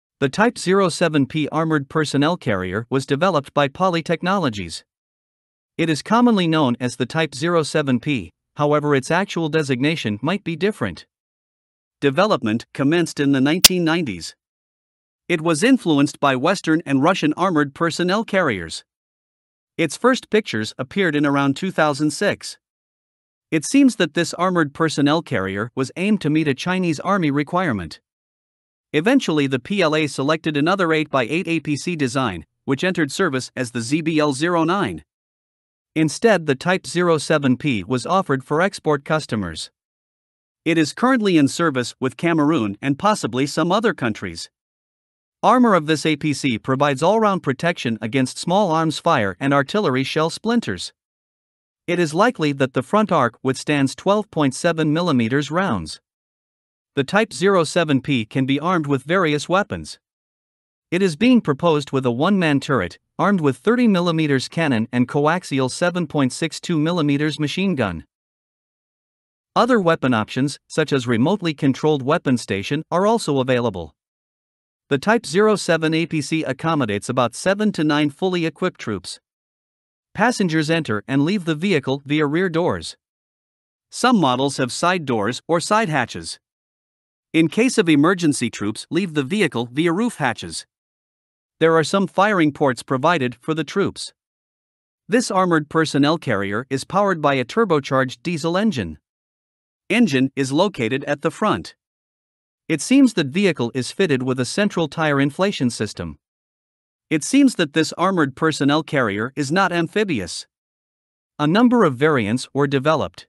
Type 07P Armored personnel carrier (China)
The Type 07P armored personnel carrier was developed by Poly Technologies. It is commonly known as the Type 07P, however its actual designation might be different. Development commenced in the 1990s. It was influenced by Western and Russian armored personnel carriers. Its first pictures appeared in around 2006. It seems that this armored personnel carrier was aimed to meet a Chinese army requirement. Eventually the PLA selected another 8x8 APC design, which entered service as the ZBL-09. Instead the Type 07P was offered for export customers. It is currently in service with Cameroon and possibly some other countries.

   Armor of this APC provides all-round protection against small arms fire and artillery shell splinters. It is likely that the front arc withstands 12.7 mm rounds.

   The Type 07P can be armed with various weapons. It is being proposed with a one-man turret, armed with 30 mm cannon and coaxial 7.62 mm machine gun. Other weapon options, such as remotely controlled weapon station are also available.

   The Type 07 APC accommodates about 7-9 fully-equipped troops. Passengers enter and leave the vehicle via rear doors. Some models have side doors or side hatches. In case of emergency troops leave the vehicle via roof hatches. There are some firing ports provided for the troops.

   This armored personnel carrier is powered by a turbocharged diesel engine. Engine is located at the front. It seems that vehicle is fitted with a central tyre inflation system. It seems that this armored personnel carrier is not amphibious.

   A number of variants were developed.